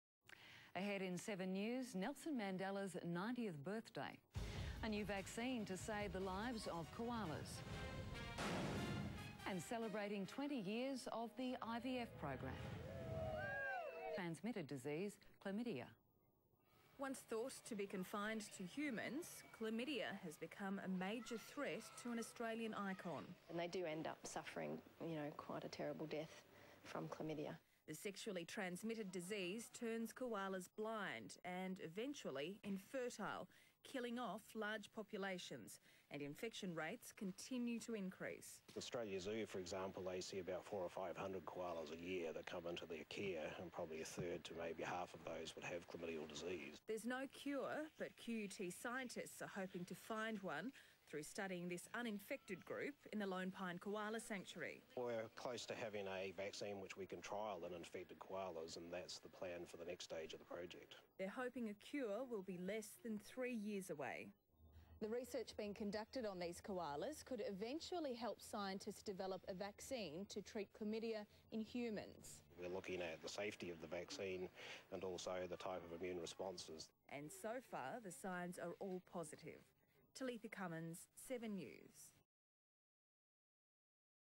QUT and Lone Pine Koala Research Project
QUT and Lone Pine Koala Sanctuary's joint Research Project on Chlamydia infection.